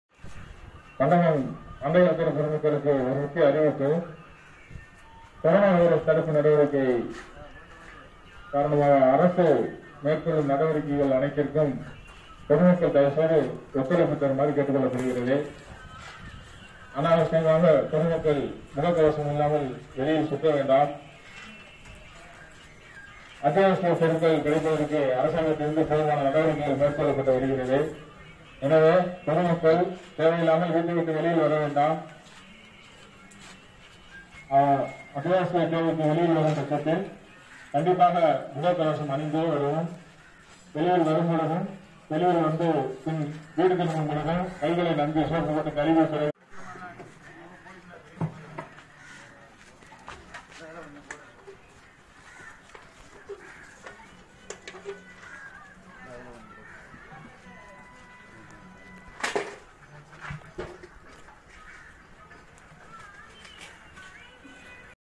ராமநாதபுரம் மாவட்டம் மண்டபம் பேரூராட்சி தண்டையல் தெருவில் கொரானா தொற்று தடுப்பு நடவடிக்கையில் தூய்மை
ராமநாதபுரம் மாவட்டம் மண்டபம் பேரூராட்சி தண்டையல் தெருவில் கொரானா தொற்று தடுப்பு நடவடிக்கையில் தூய்மை பணி தீவிரப்படுத்தப்பட்டுள்ளது.